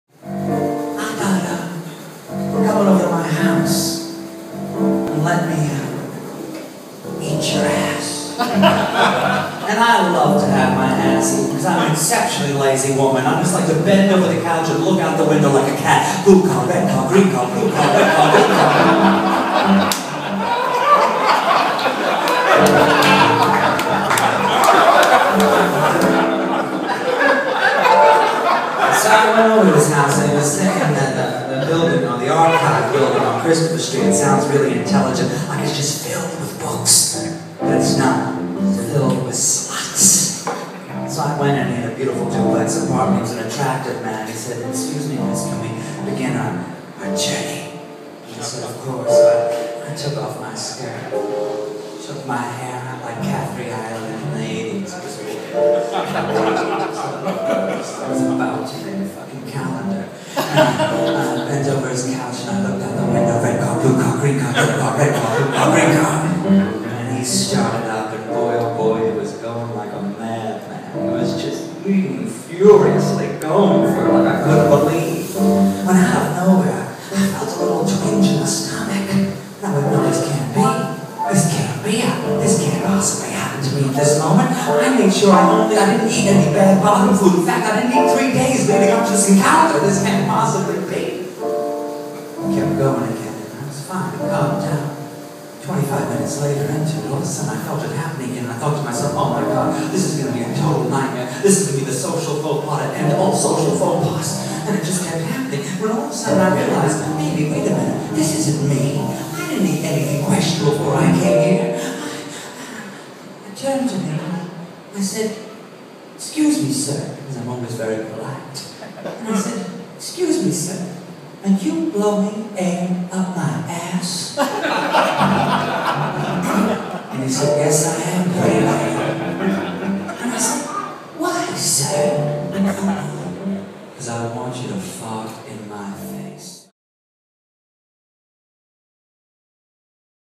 Hedda Lettuce and the Awkward Rimjob
A small preview from Hedda Lettuce's one-woman drag cabaret, "The Carpets Match the Drapes," performed live at XL Nightclub in New York City.